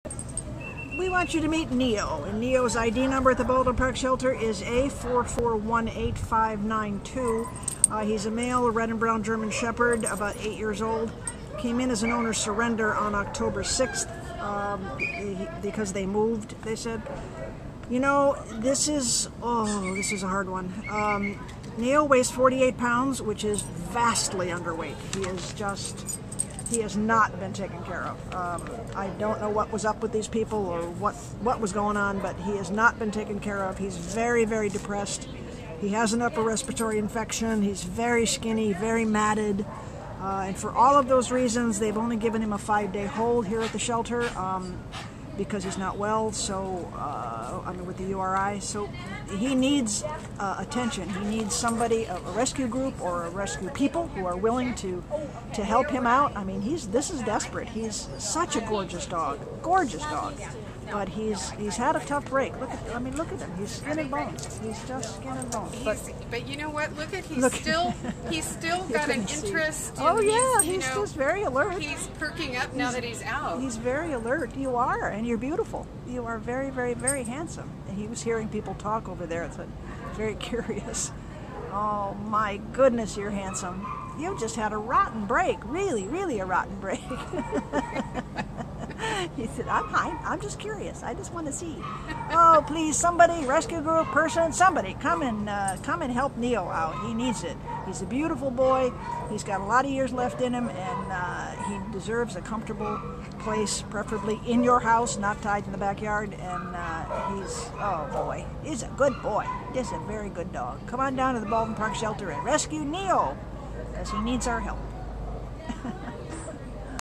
A4418592 Neo | German Shepherd
A4418592 Neo is an endearing 8-year-old caramel-and-black male German Shepherd whose owners abandoned him at the Baldwin Park Animal Care Center on October 6th because they were moving and didn’t take him along. Weighing about 50% of what he should weigh—he’s at 48 lbs and should be 90+—and showing other signs of neglect, this gentle giant is nonetheless alert, loving, and eager to be with and please people. He’s fine on leash, shows signs of housebreaking, has a low energy level (to be expected given his lack of health), and is fine with other dogs. What’s most remarkable about Neo, though, is that his nobility and fine spirit shine through all his current ailments—wobbly back legs, emaciation, an upper-respiratory infection (that’s being treated), and depression. We’re hoping that Neo will be rescued or adopted by a compassionate person or family who wants to see him brought back to health and understands that he’s super-worth having as a beloved and loving inside pet and family member.